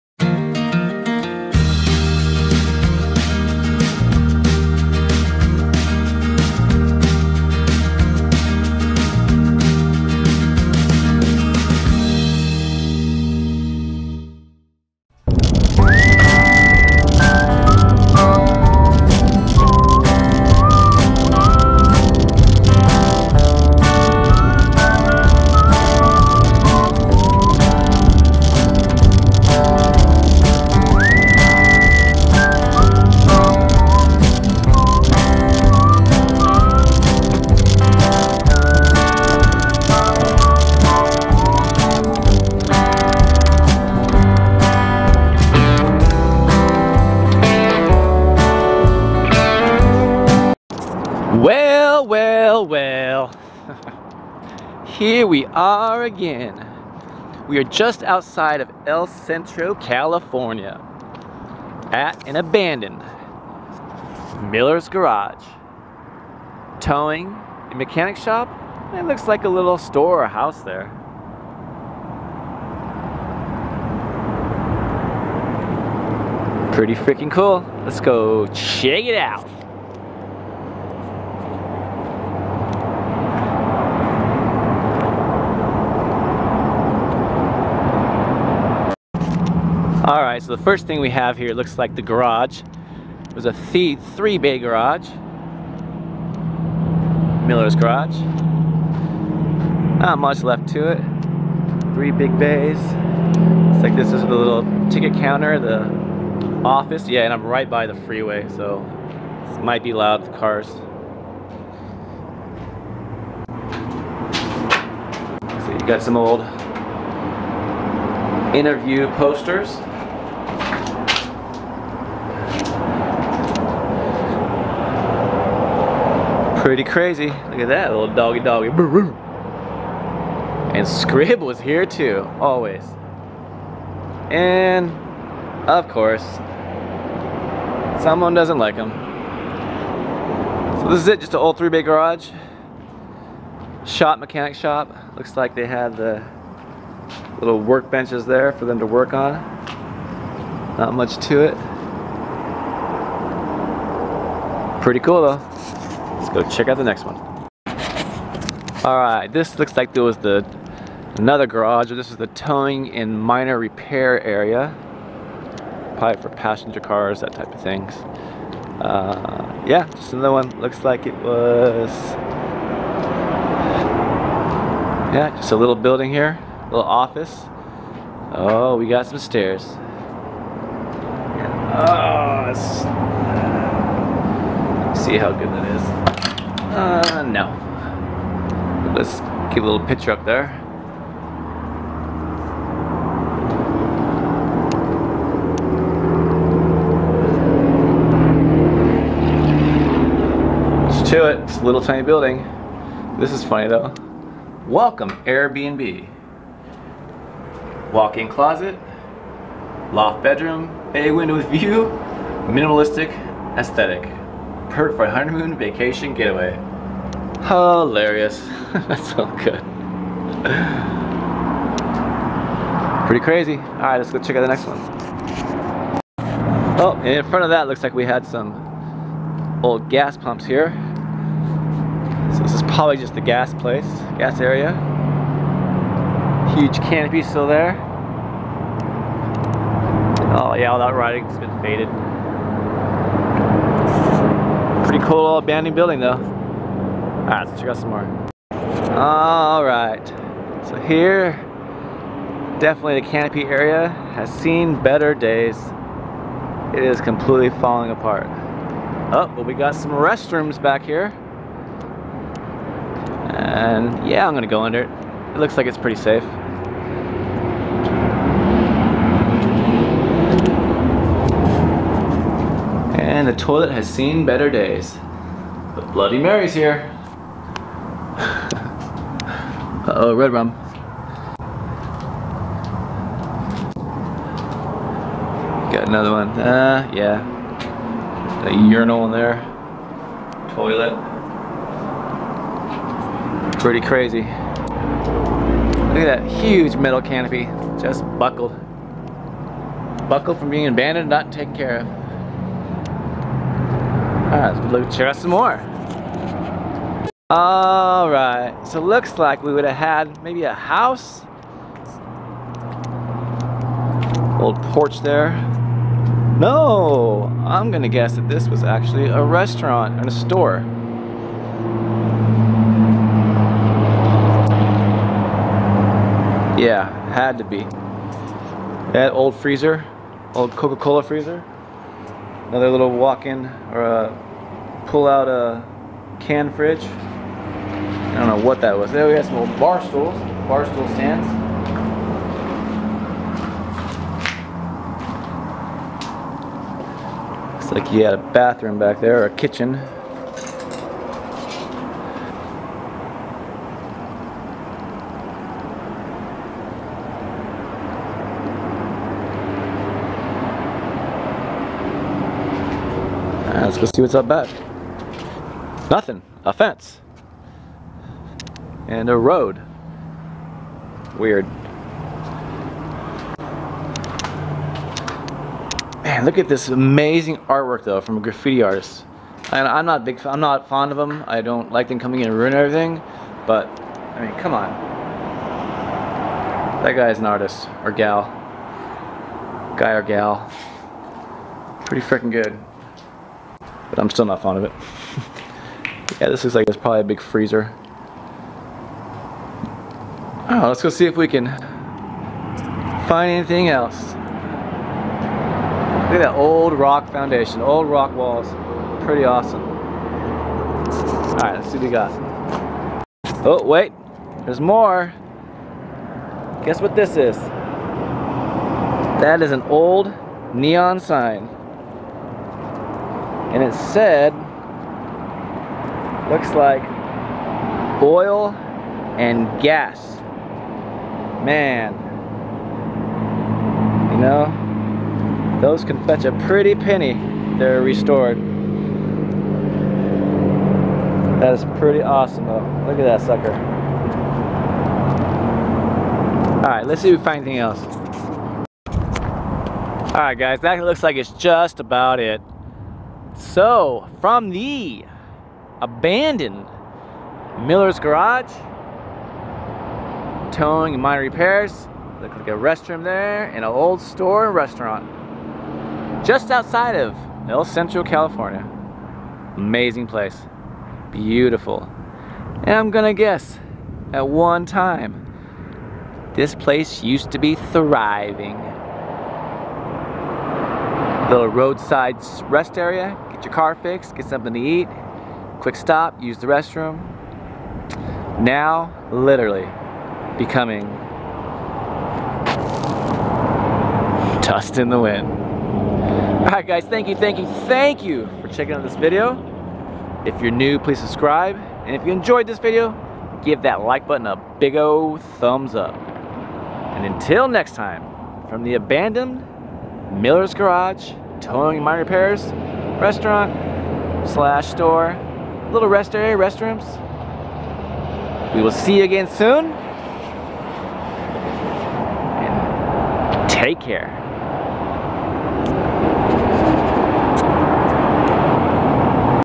Abandoned!!! Millers Garage/Towing/Rest Area/Store!!! California!!
Checked out the Abandoned Millers Garage/Towing/Rest Area/Covenience Store!!! Located just out of El Centro, CA...  This place I'm sure was owned by a family, and used to be THRIVING at one point... Decaying in the California desert these days...